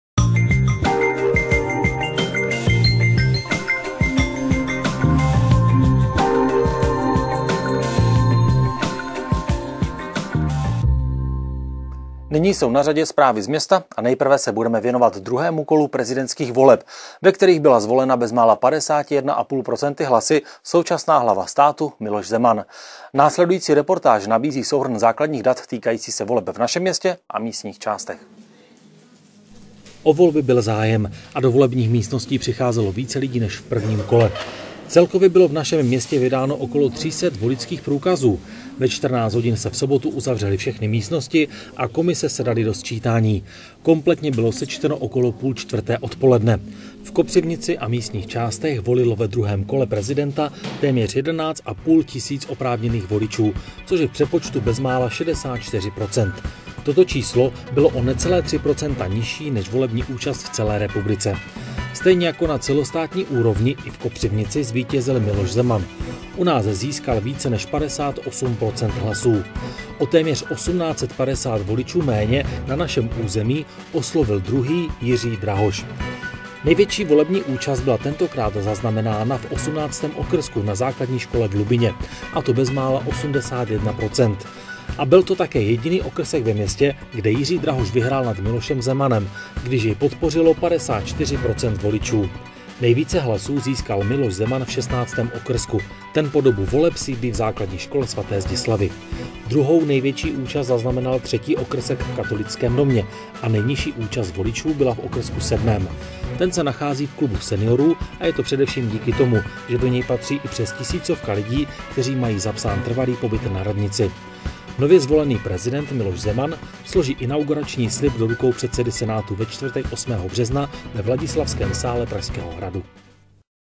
KTK studio - 2. 2. 2018, Magazín č. 3 - VOLBY 2018, prezident, 2 kolo
Ve druhém kole prezidentských voleb v Kopřivnici volilo téměř 64 % voličů, což je více než v prvním kole.